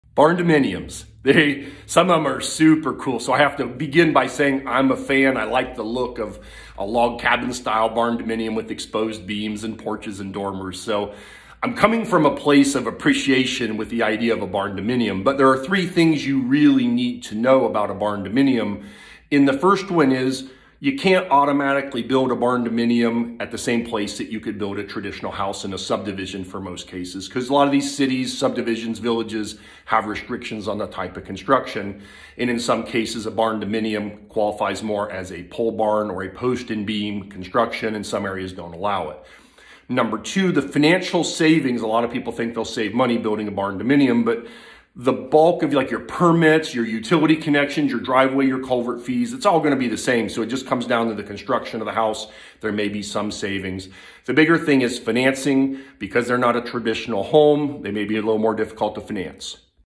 3 Things to know about Barndominiums
I love the look of a lot of the barndominium style homes especially when they have a log home vibe with a country front porch.  The openness of a lot of these types of homes reminds me of a feeling you get when you visit a lodge.  A lot of people are attracted to the barndominium because of the style but I am betting even more consider them because of the belief that they can save money due to the different type of construction that typically comes with a barndominium.

It's important that I share with you that I am a fan of different types of construction when it comes to homes and housings and there are a lot of barn homes that I think are awesome.  However, there are 3 things that really stand out that I think a lot of people don't think about when they are planning to build a barndominium.

1.  A barndominium utilizes in most cases a different type of construction than your typical 2x4 or 2x6 conventionally built home that most of us grew up in.  Barn homes generally are built using either a pole or post construction depending on the size and budget.  Because they are built differently, they don't automatically fall into the same category as a traditional home construction category and this is where things can become a little more complicated.  For example, there are restrictions within some cities, housing subdivisions, developments, villages and even townships that restrict certain types of home design and construction types so make sure you check BEFORE you commit to a barndominium.

2.  Price!  There are a lot of people who seek out a barndominium with the belief that they are going to be able to build a barndominium like they would build a garage or pole barn and save a ton of money.  This isn't usually the case and if you don't plan well you could end up spending MORE building a barndominium than a conventionally constructed home especially if your contractor has never built a barndominium before.

3.  Financing!  Barndominiums don't always fall under the same category as a conventionally built house and some loans or financing types don't automatically approve barn homes.  There is another element of this to think about and that is that an appraisal is a big part of the mortgage process.  If the appraiser can't find 3 or 4 similar type constructed homes (barn homes) to use as comparable for the appraisal, that may become an issue for you when it is time to obtain your loan.  This can become a BIG issue when it comes to sell your barndominium because if it doesn't appraise or if your barndominium doesn't meet the banks loan requirements, they won't likely approve the new buyer for the purchase of your barndominium.

With all this said, we can't deny the attraction of a barn style home especially one that is done well.

Sam Miller
REMAX Stars Realty
710 South Main Street
Mount Vernon, Ohio 43050